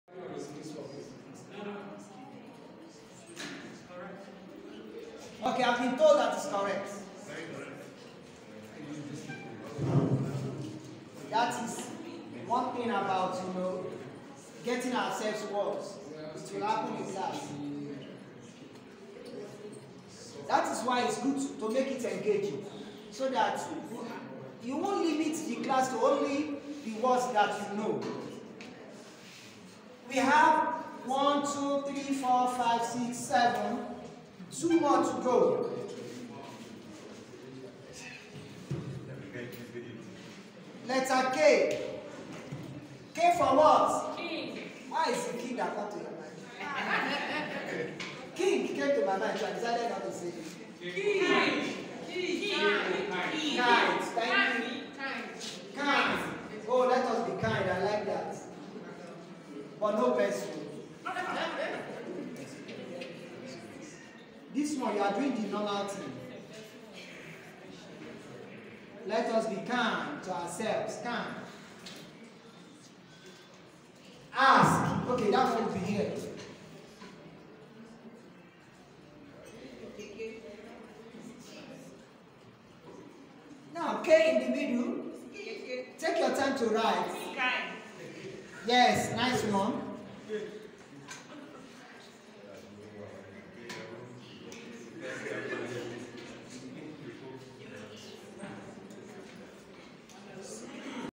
Nelson Handwriting Training for Teachers of Sheikh Abubakar Gummi Academy, Abuja Nigeria.
How to Teach Flick Letters with Word Formation